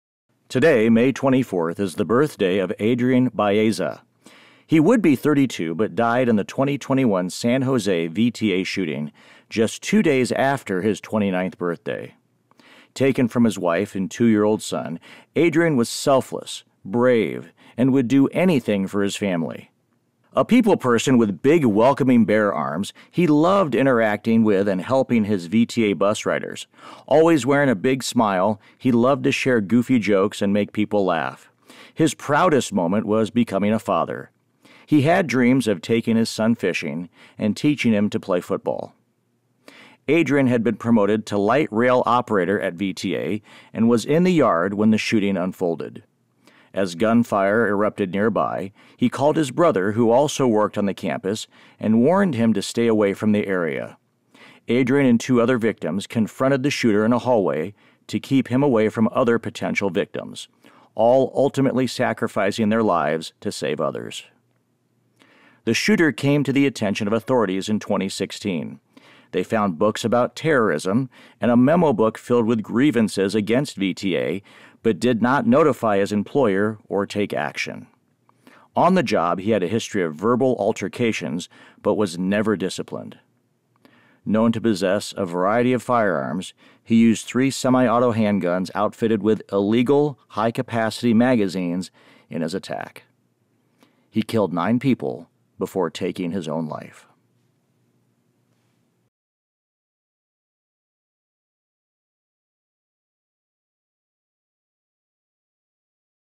May 24 Adrian Balleza
Today, May 24, is the birthday of Adrian Balleza. He would be 32 but died in the 2021 San Jose VTA shooting, just 2 days after his 29th birthday. Taken from his wife and 2-year-old son, Adrian was selfless, brave, and would do anything for his family.

A people person with big welcoming bear arms, he loved interacting with and helping his VTA bus riders. Always wearing a big smile, he loved to share goofy jokes and make people laugh. His proudest moment was becoming a father. He had dreams of taking his son fishing and teaching him to play football.

Adrian had promoted to light rail operator at VTA and was in the yard when the shooting unfolded. As gunfire erupted nearby, he called his brother who also worked on the campus and warned him to stay away from the area. Adrian and 2 other victims confronted the shooter in a hallway to keep him away from other potential victims, all ultimately sacrificing their lives to save others.

The shooter came to the attention of authorities in 2016. They found books about terrorism and a memo book filled with “grievances” against VTA but did not notify his employer or take action. On the job, he had a history of verbal altercations but was never disciplined. Known to possess a variety of firearms, he used 3 semi-auto handguns outfitted with illegal high-capacity magazines in his attack. He killed 9 people before taking his own life.